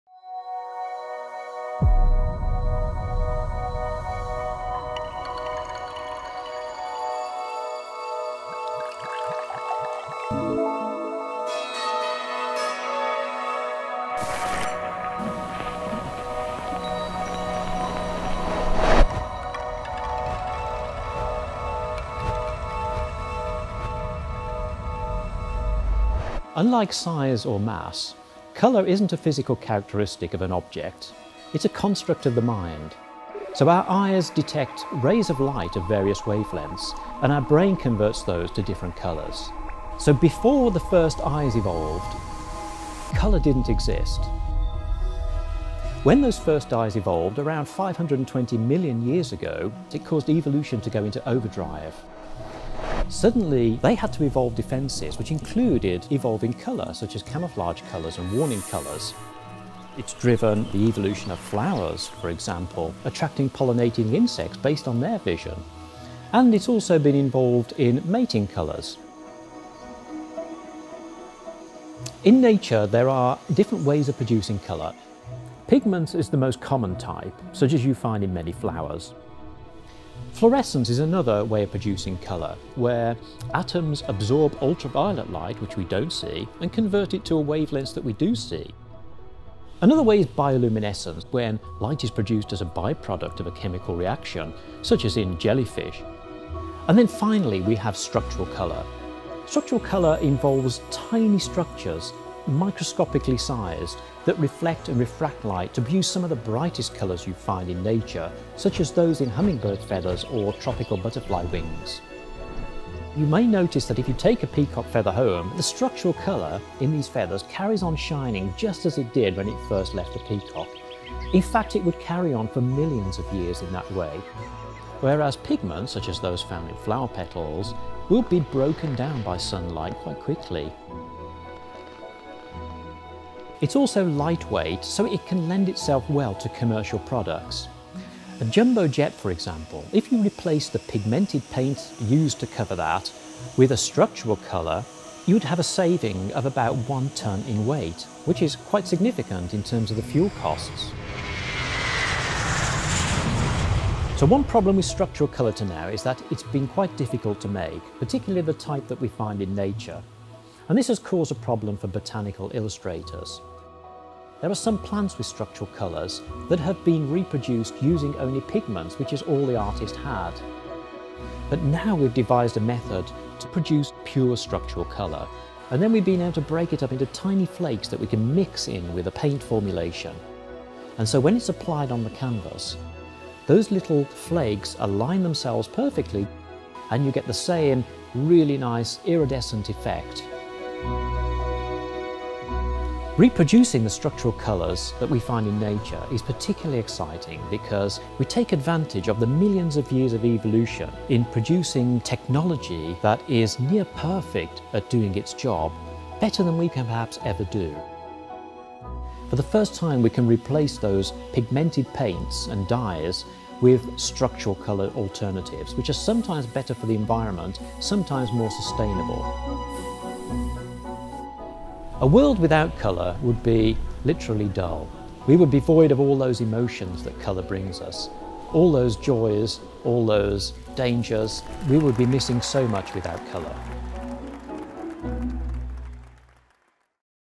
What is colour?
Pure Structural Colour is the boldest, brightest colour in the natural world. Colour brings the natural world to life - but isn't a physical characteristic of an object. It needs light to be seen.

The artist, scientist, and best-selling author Andrew Parker takes on a journey of colour throughout evolution and how structural colour can be replicated by humans now.